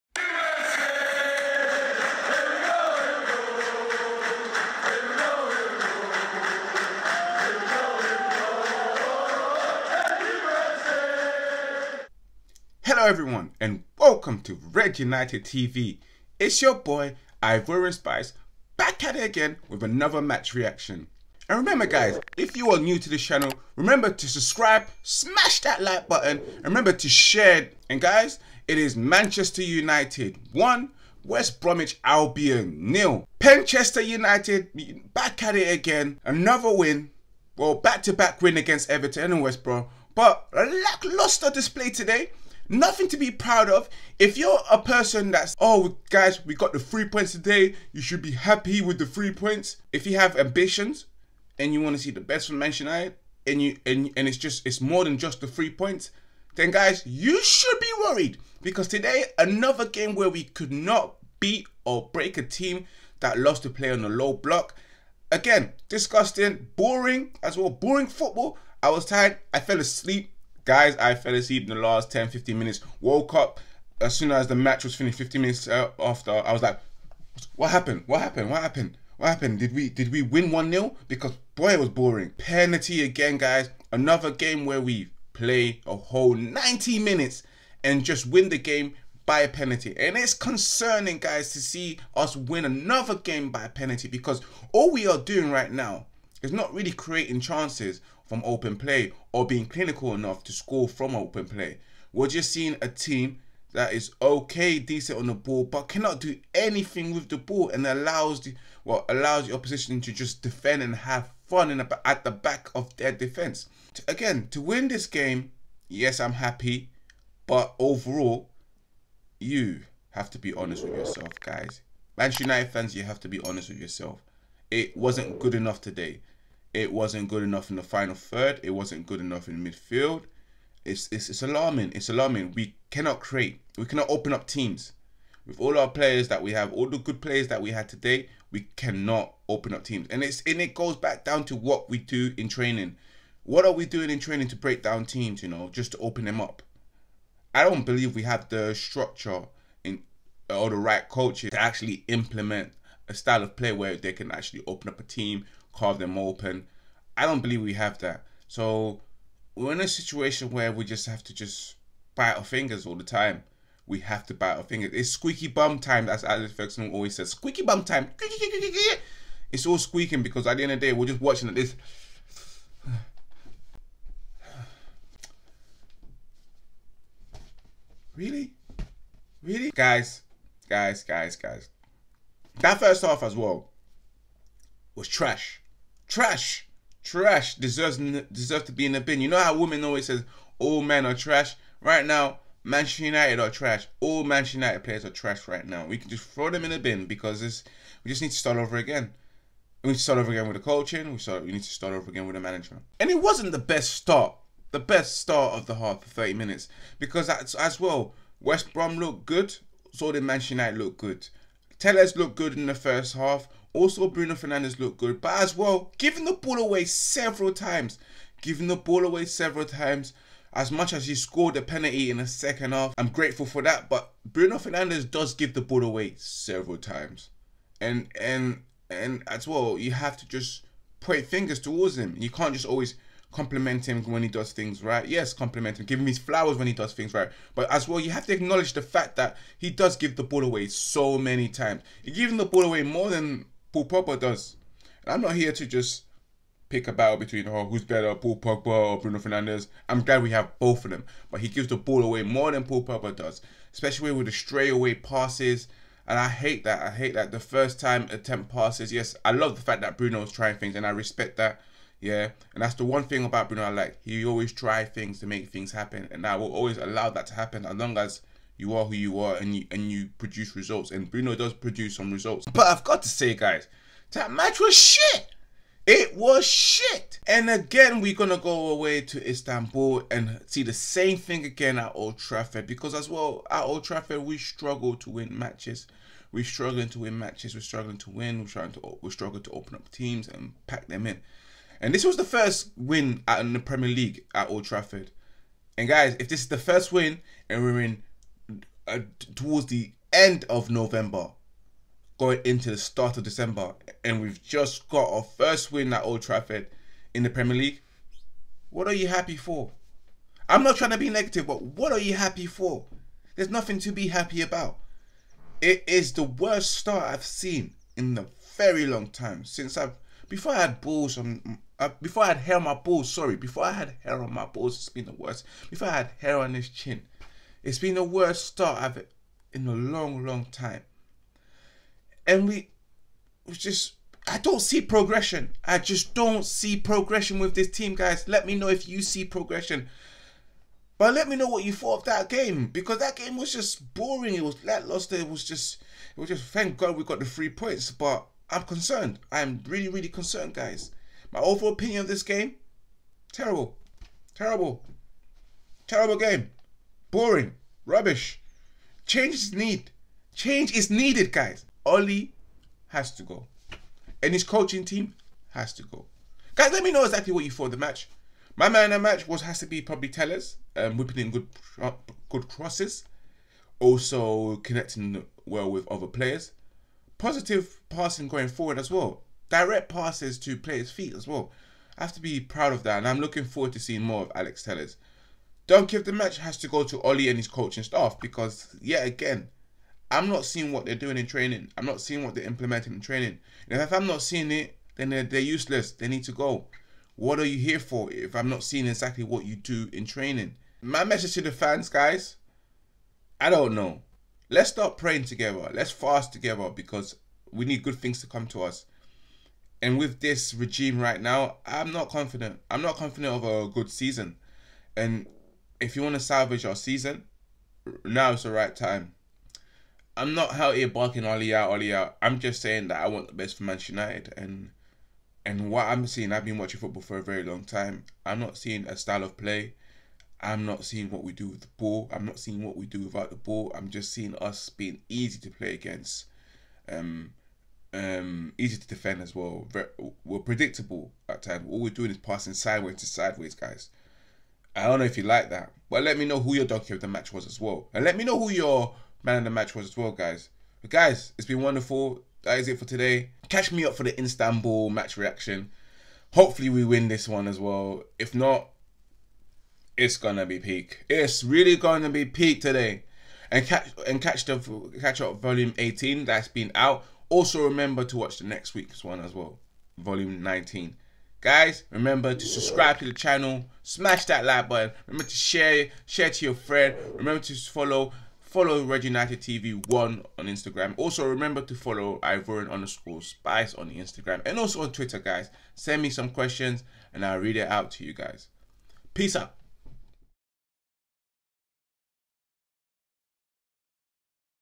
Man United vs West Brom | Match Reaction
Match reaction of the Manchester United against West Brom on 22.11.20 Premier League match.